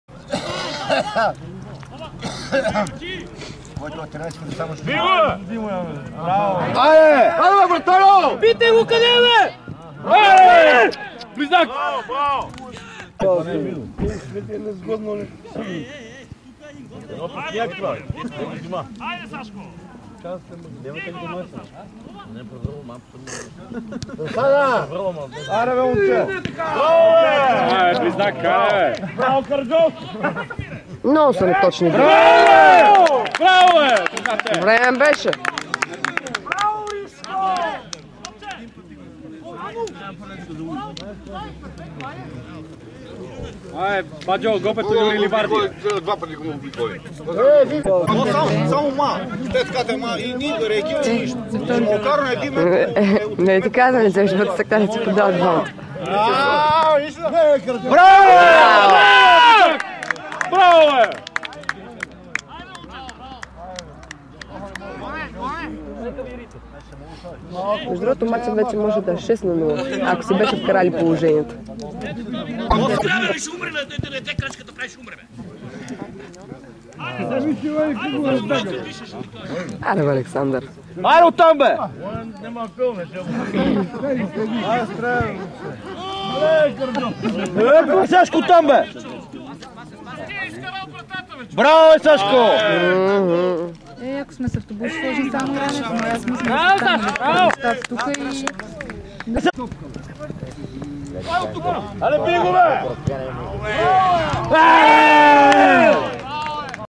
Ерден 0:4 Вършец 2012
samo 2 ot golovete sa vklucheni drugite sa bezsledno izcheznali :D